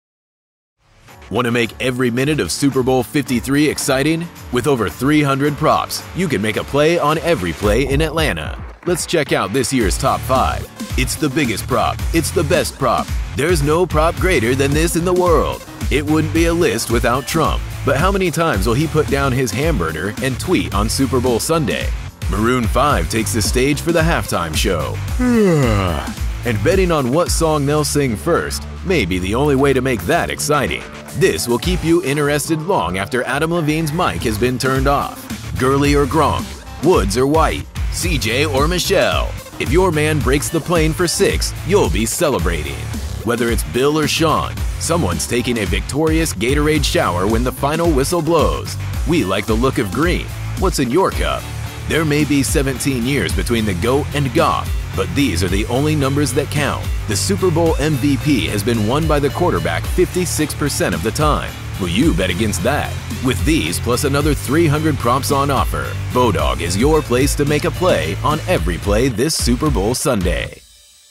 Super Bowl 53 - Bodog's Top 5 Props
With over 300 Super Bowl 53 Props available to bet on, Bodog has the largest and widest selection available in Canada. We count down our Top 5 props to bet on as the Rams take on the Patriots.

Have a favourite prop? Make a play by checking out all of our props at Bodog.